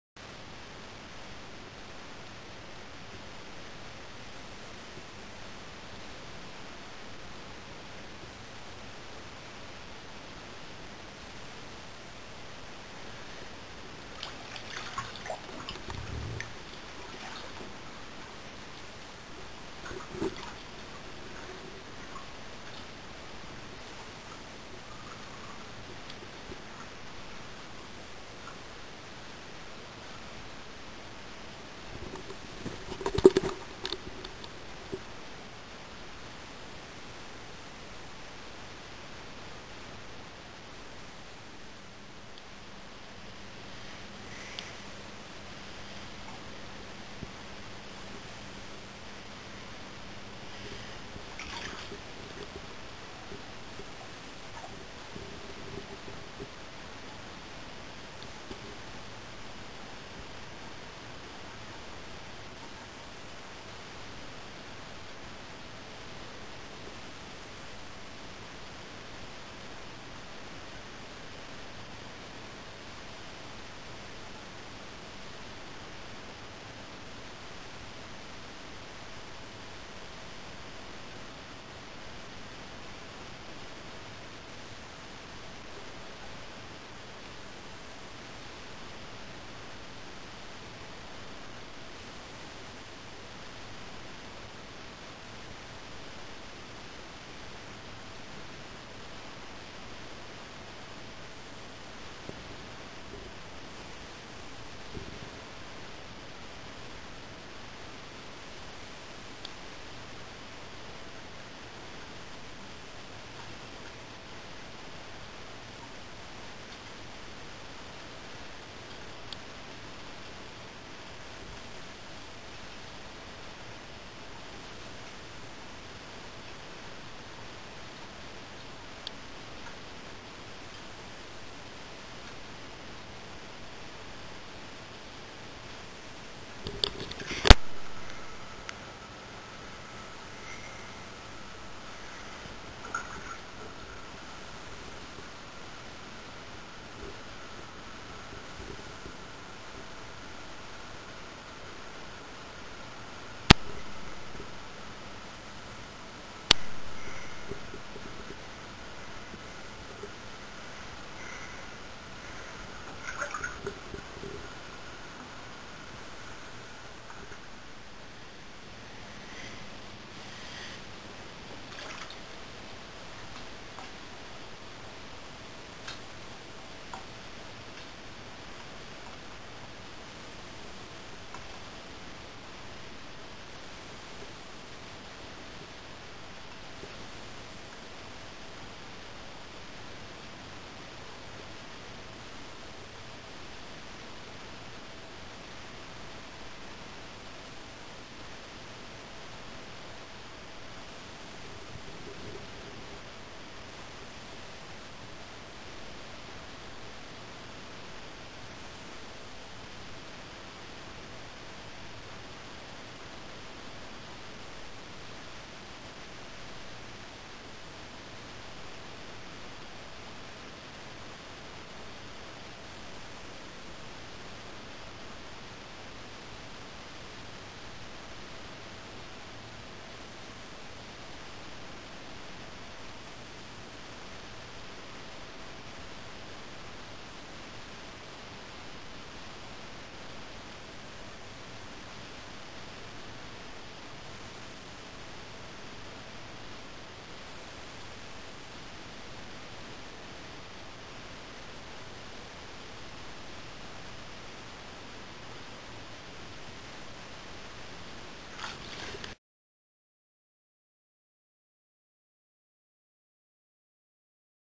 breathhold in bath (first tries)
well a lot of people wanted me to make a video of holding my breath in the bathtub... and to hold it as long as i can. so i tried it a few times and these are the first tries and the last one is in another video (my breathholding record 1 minute and 32 seconds). and sorry becasue its very dark but we don't have that many lights in the bathroom :) and i didn't know its gonna be so dark on the video.